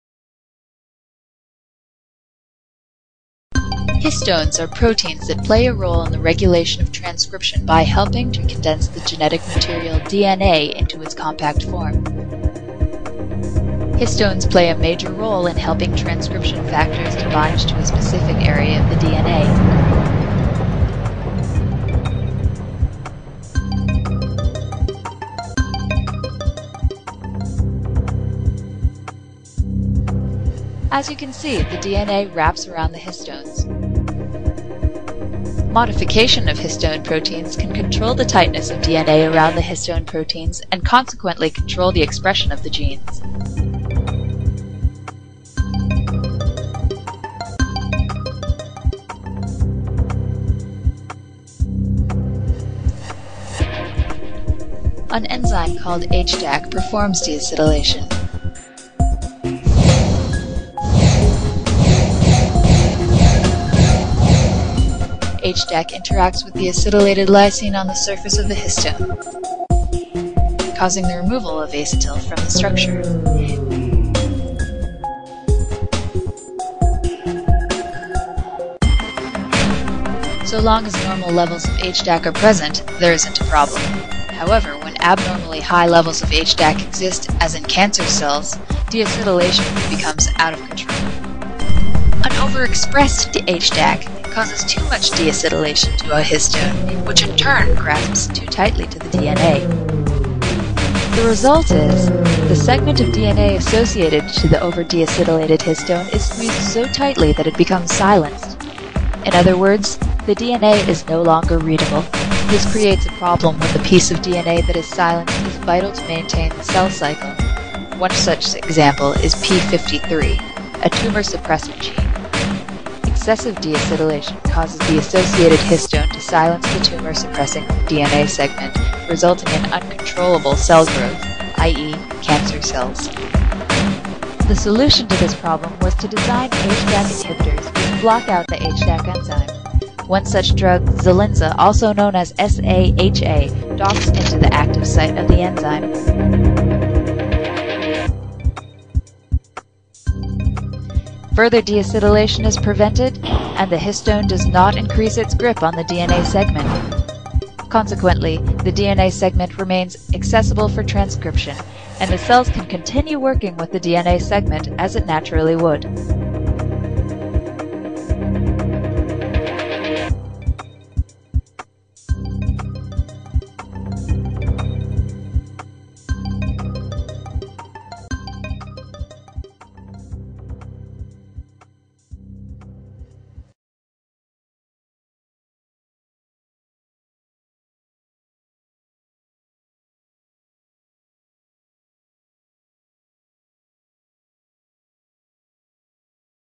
Histone Deacetylation
Deacetylation removes acetyl groups from histone tails, causing the histones to wrap more tightly around the DNA and interfering with the transcription of genes by blocking access by transcription factors. The overall result of histone deacetylation is a global (non specific) reduction in gene expression. Histone Deacetylase involved in a series of pathway in the living system.